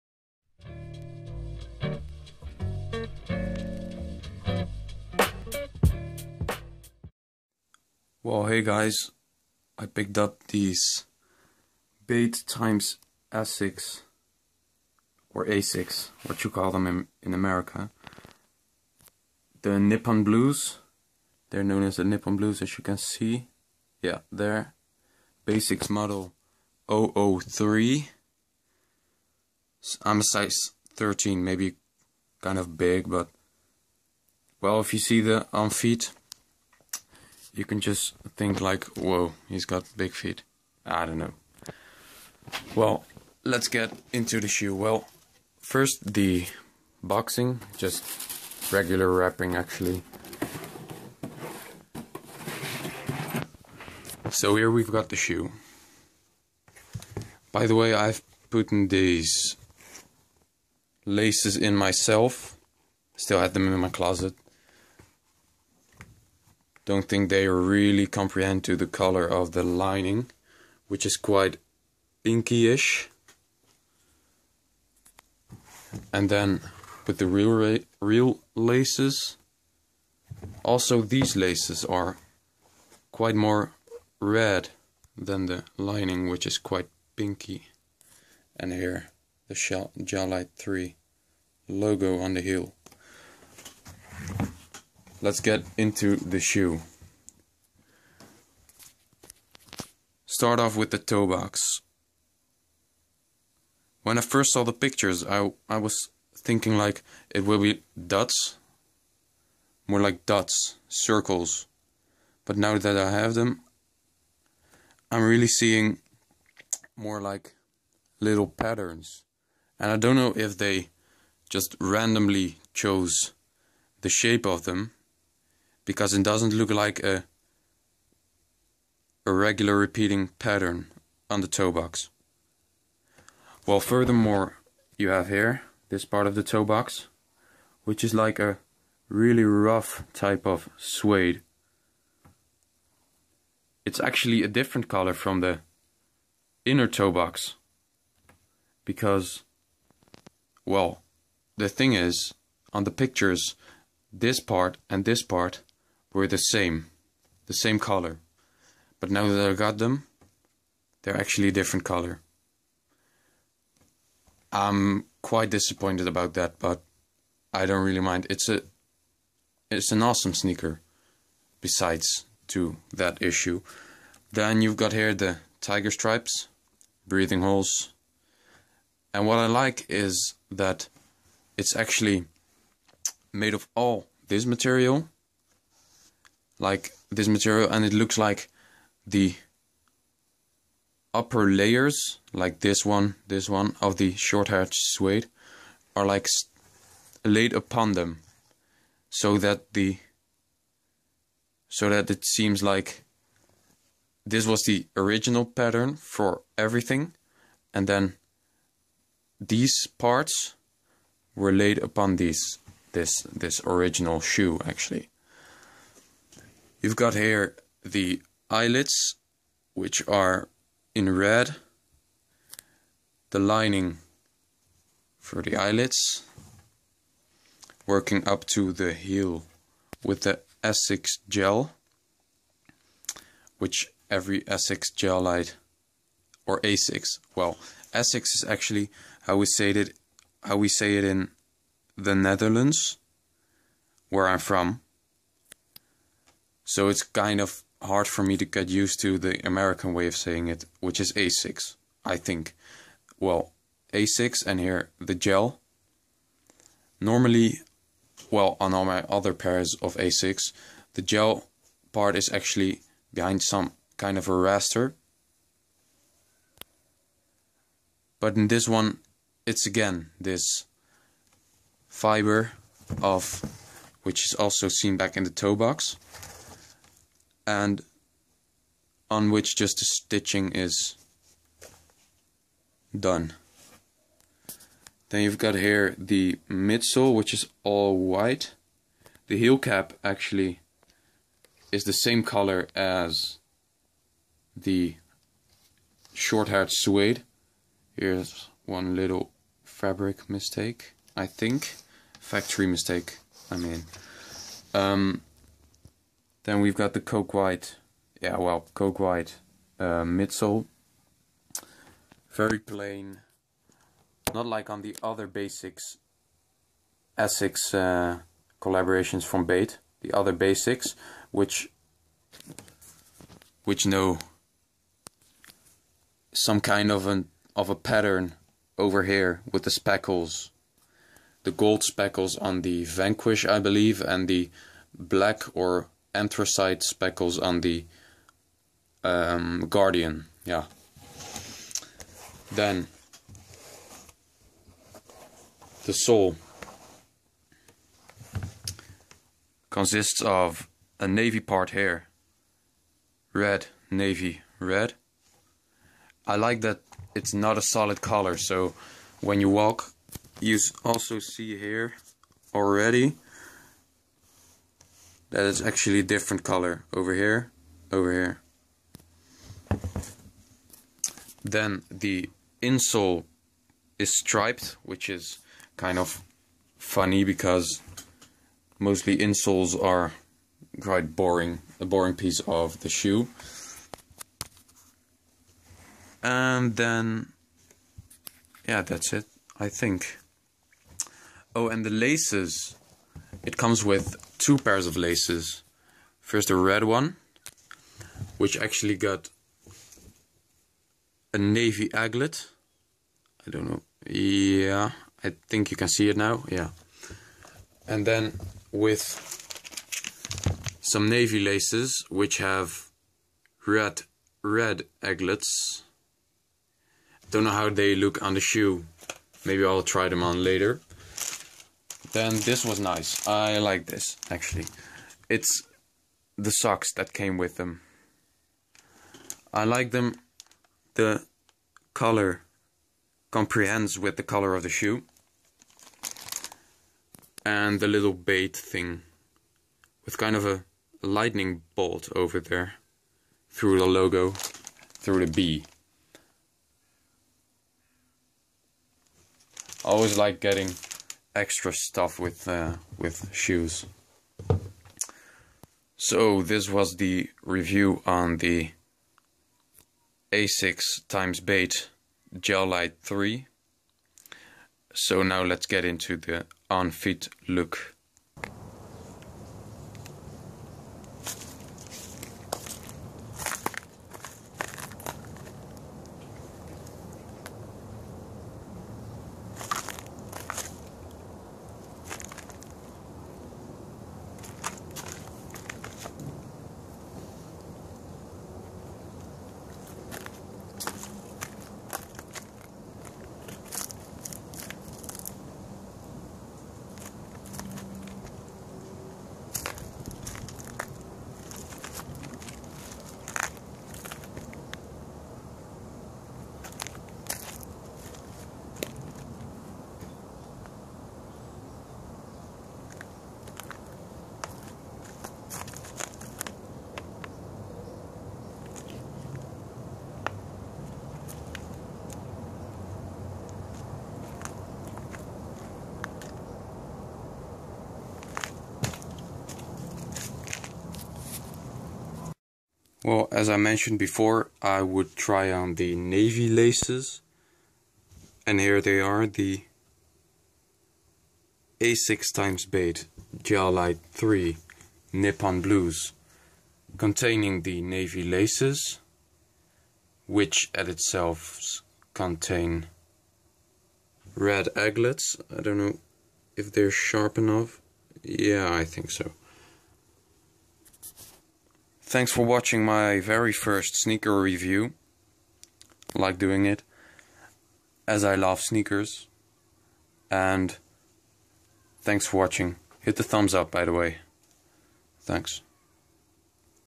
Asics x Bait Gel Lyte III 'Nippon Blues' Review and On-Feet 1080p HD
Here's a quick review and on feet of my newly picked up Asics x Bait Gel Lyte III 'Nippon Blues'.
Sorry for my bad English, I'm not a native and didn't prepare any text, promise to do better with my next pick-up.